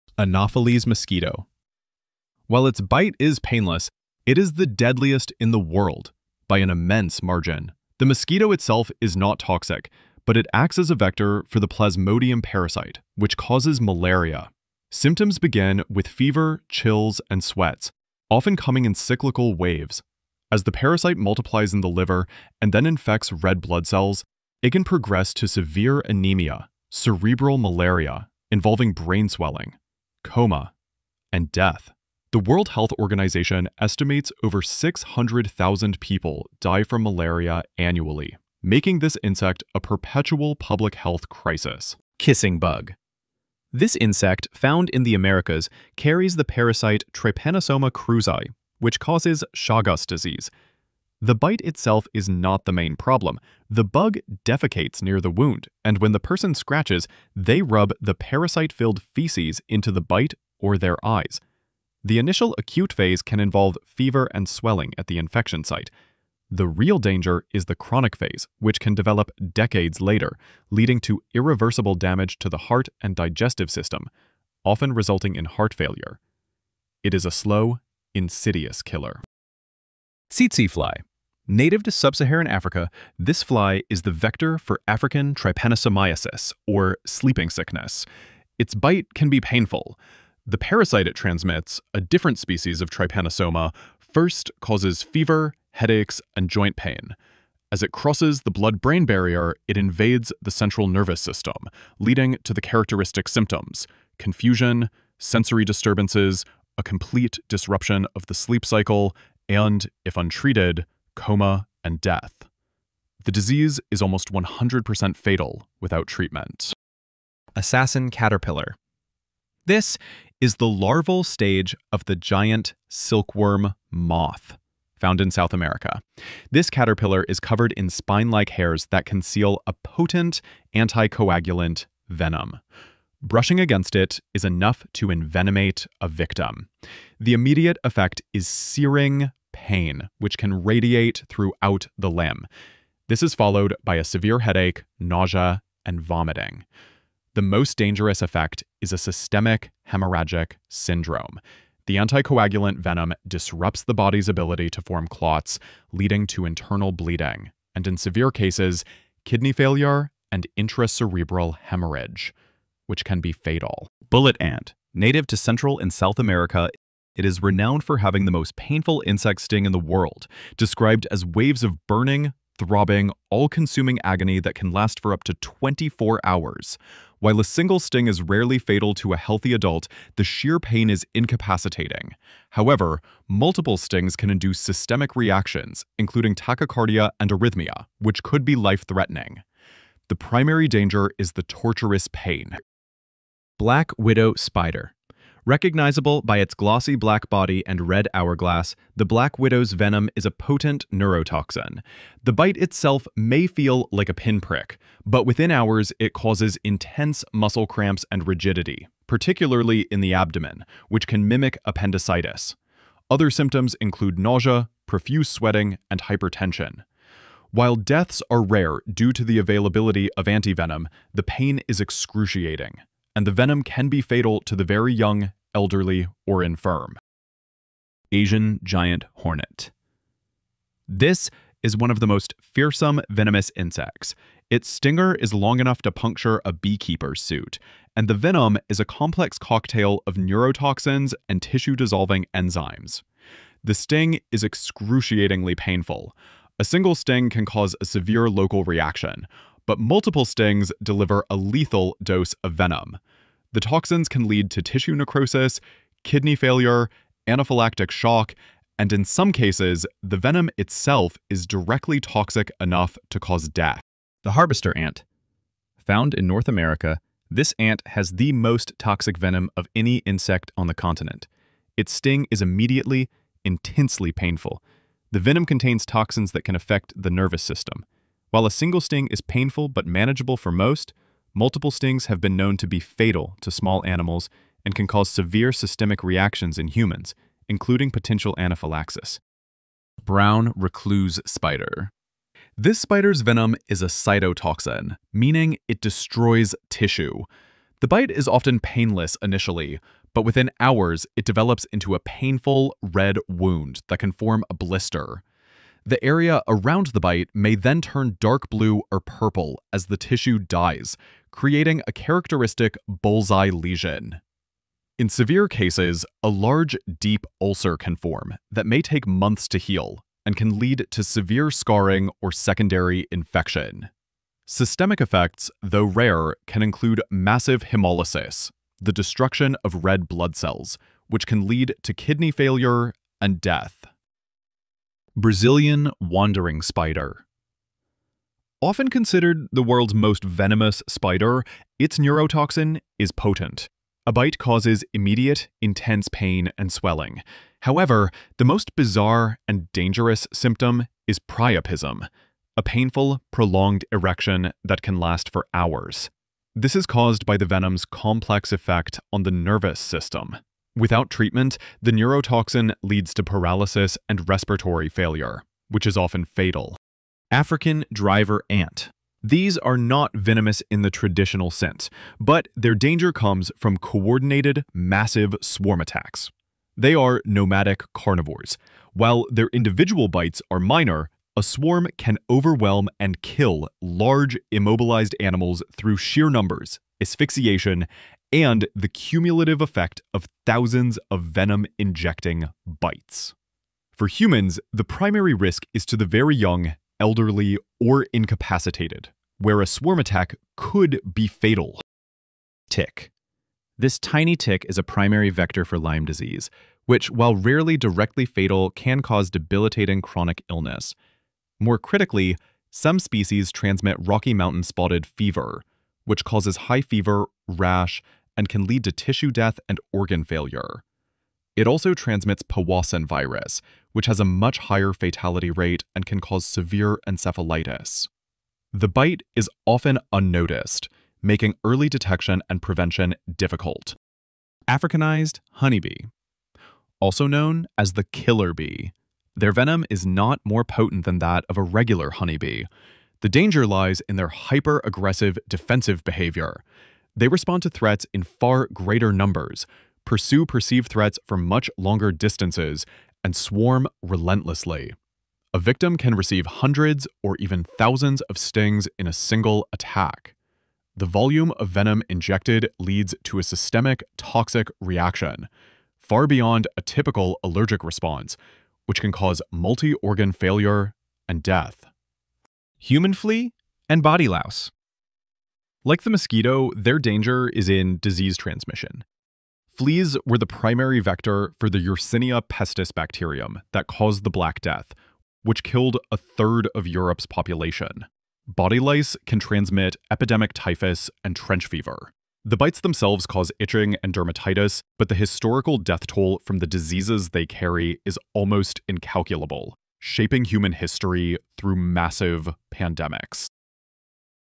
The Deadliest Insect and Arachnid Bites in 8 Minutes
Ever wondered what the Deadliest insect and spider bites you could experience? This video breaks down the Deadliest Insect and Arachnid Bites in 8 Minutes!

This video is for entertainment and educational purposes only and some information may not be fully correct. You are encouraged to do your own research to gain better understanding.